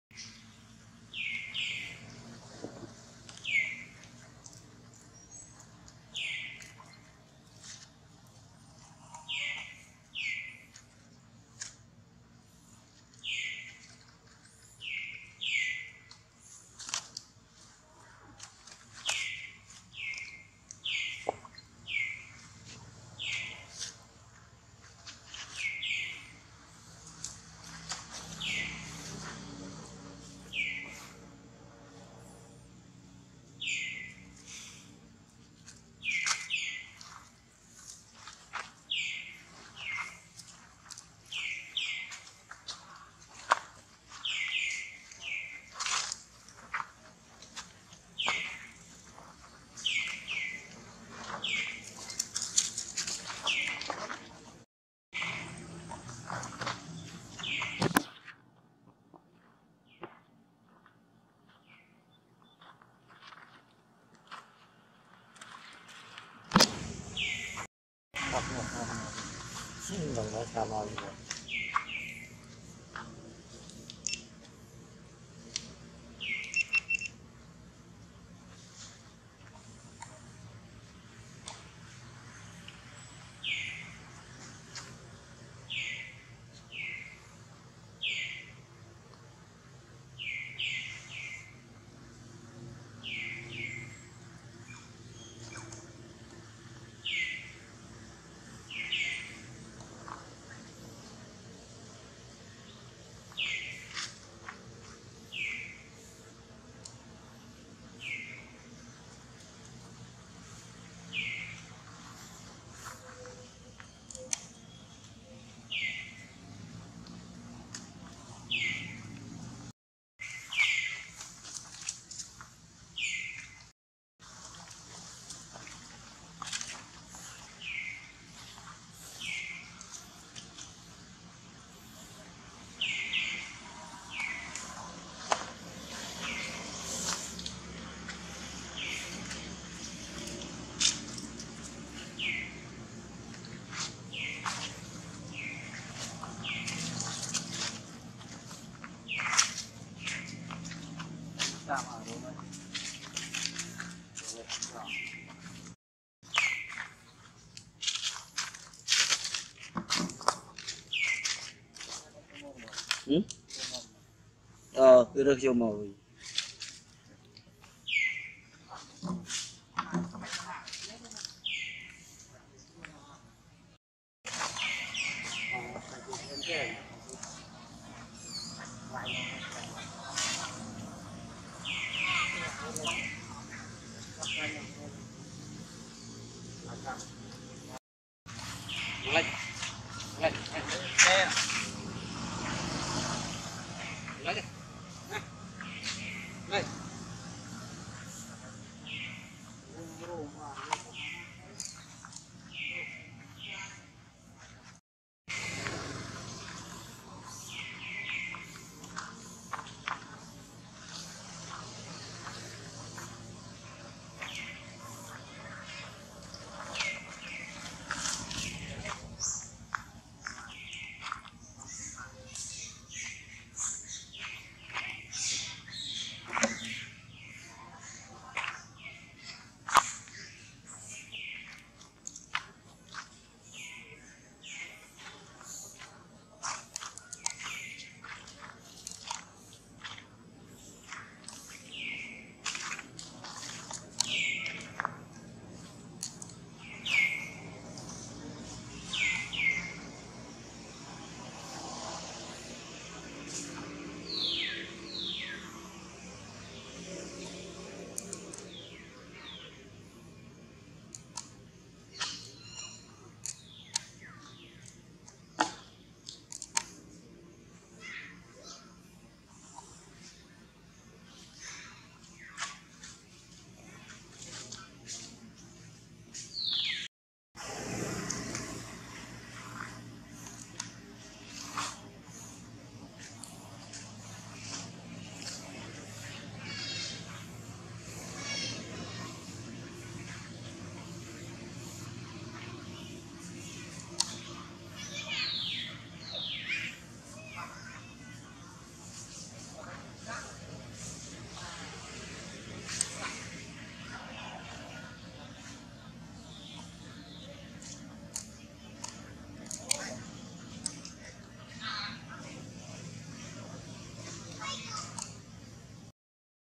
Hot News! Welcome Back to Amber Troop Mr Rolex After Missed For a long time.
Hot News! Welcome Back to Amber Troop Mr Rolex After Missed For a long time, Rolex Happy To Sweet home.